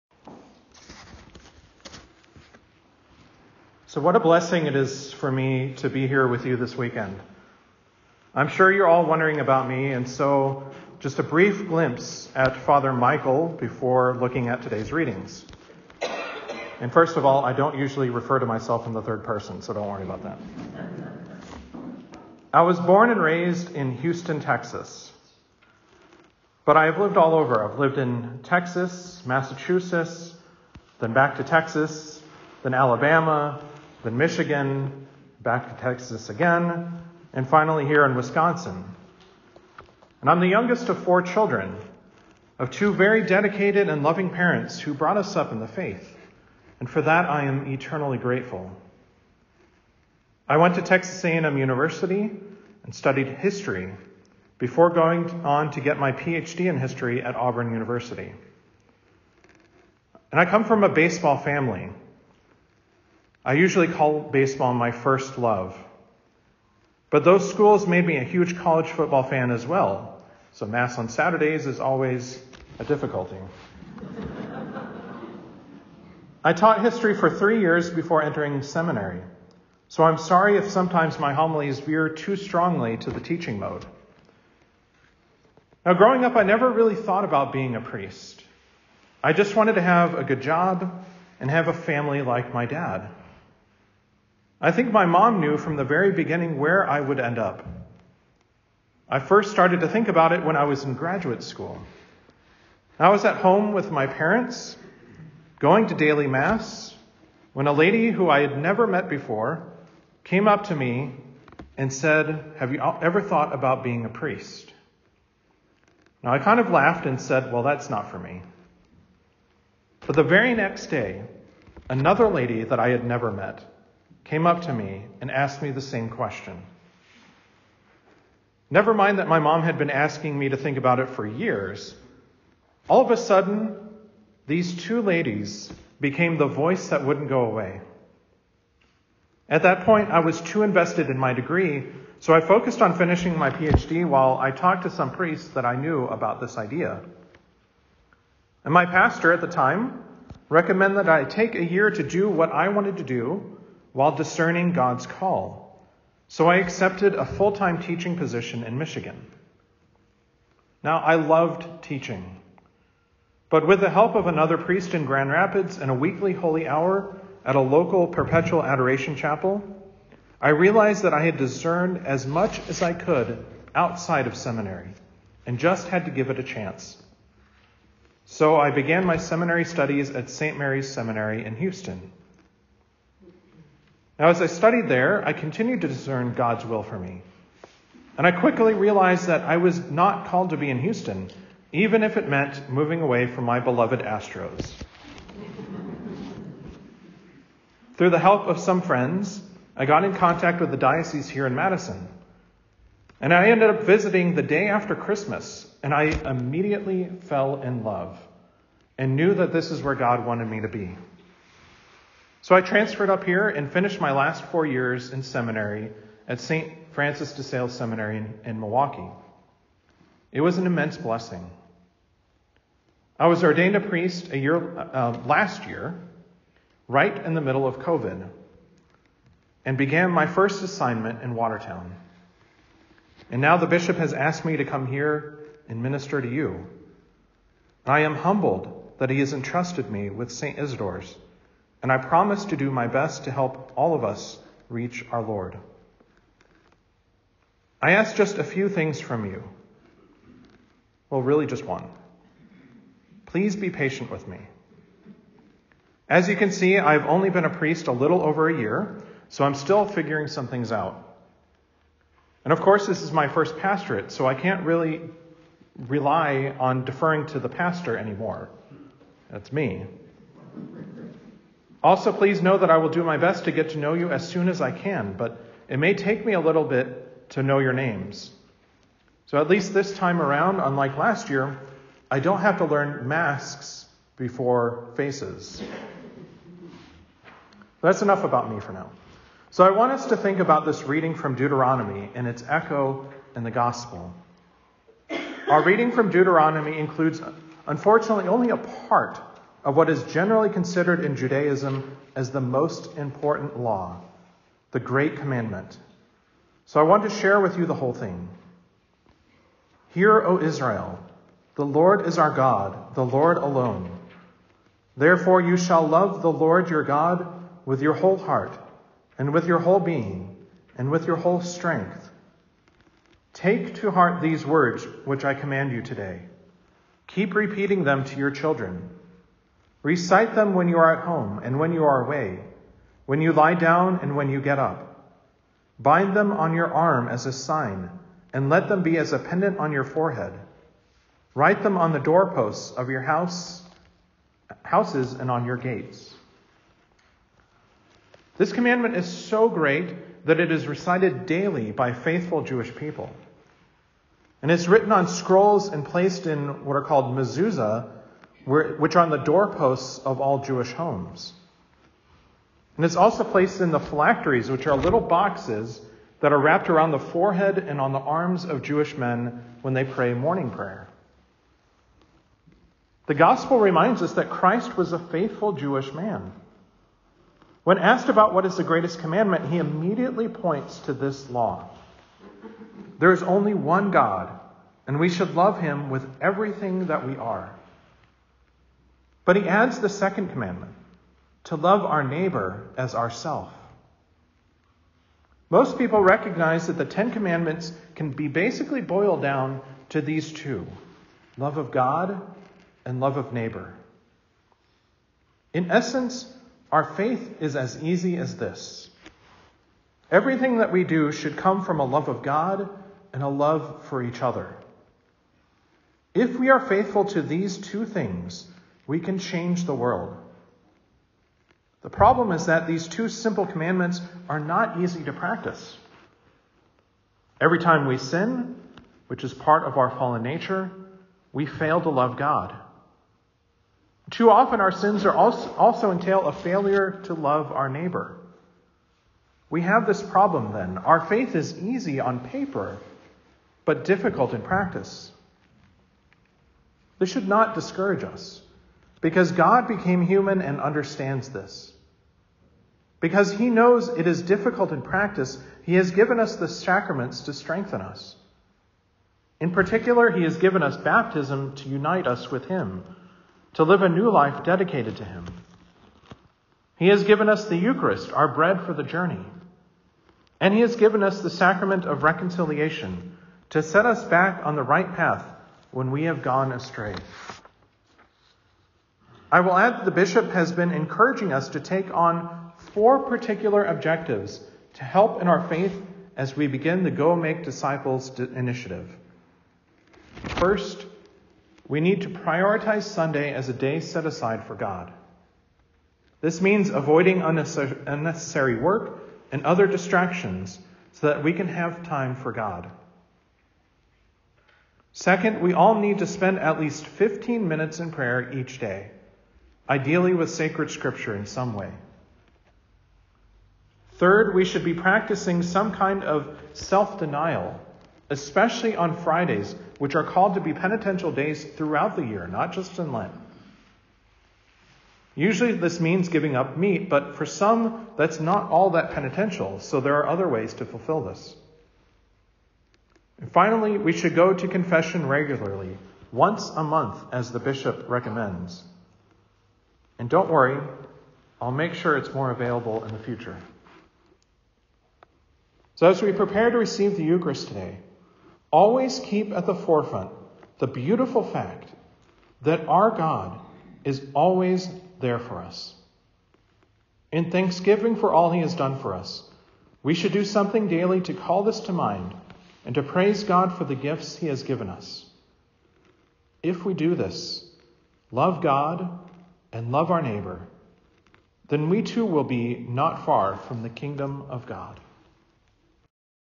Homily for the 31st Sunday of Ordinary Time, 10/30/2021 (Fr. Michael First Homily)
First Homily of Fr. Michael at St. Isidore's Parish
31st Sunday of Ordinary Time, Year B